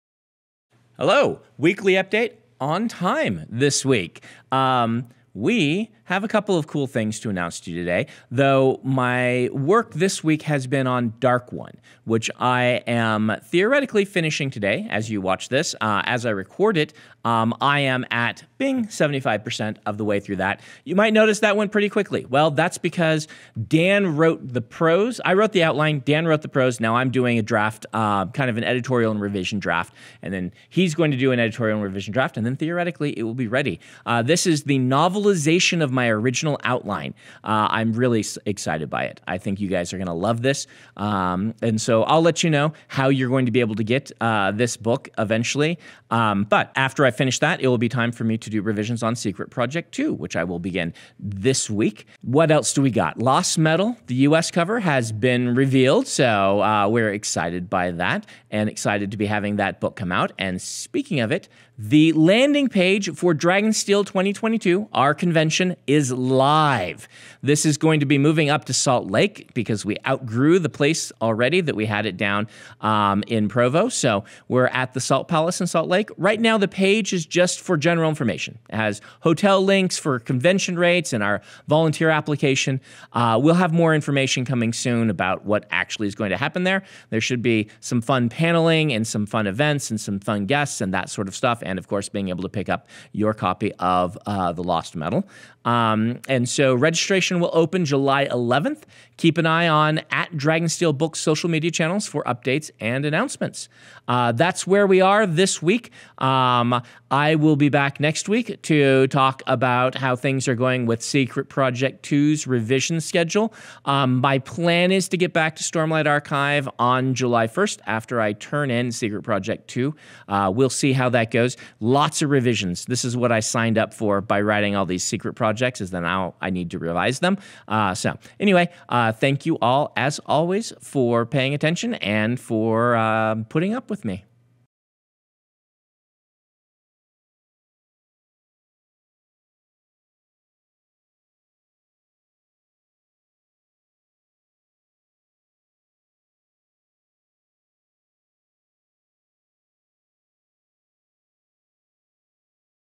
Dragonsteel 2022 + Weekly Update
Welcome to my continuing update series where I give you a breakdown of the previous week.

Want to send me something to open?
Dragonsteel Entertainment
ATTN: Adam